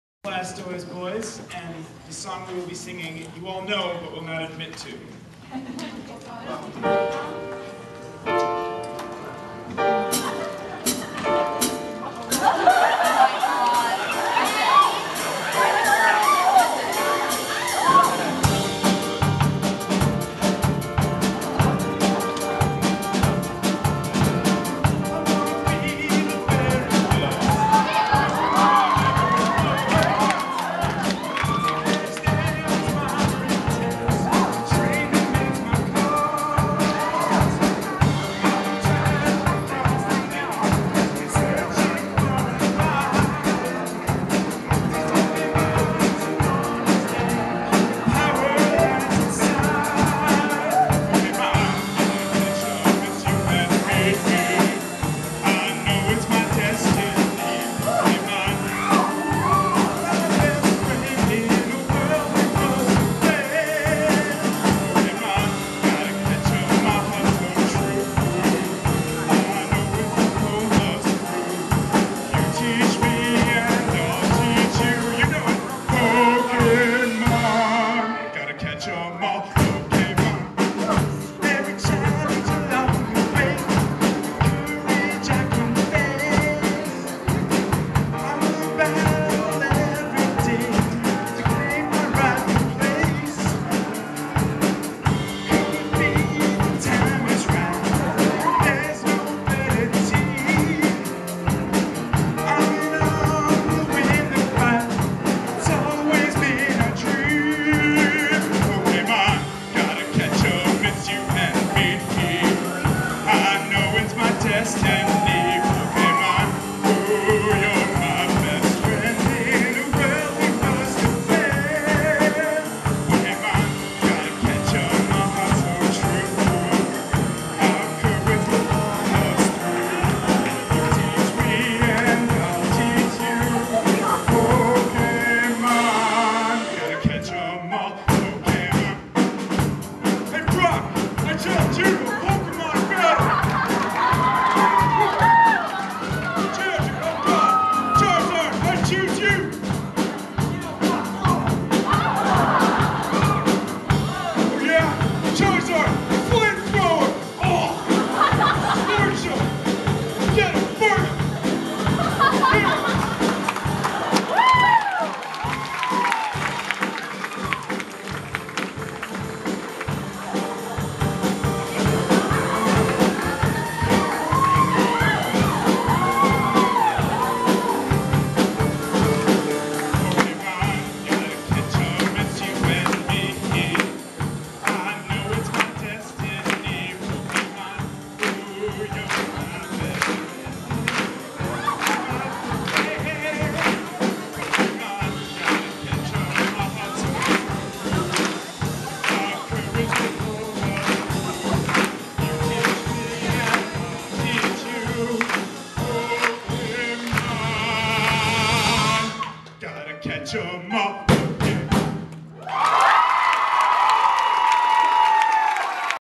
The Blastoise Boys
Blastoise Boys performing during the Boy Band Competition 2010 at UW-Madison Summer Music Clinic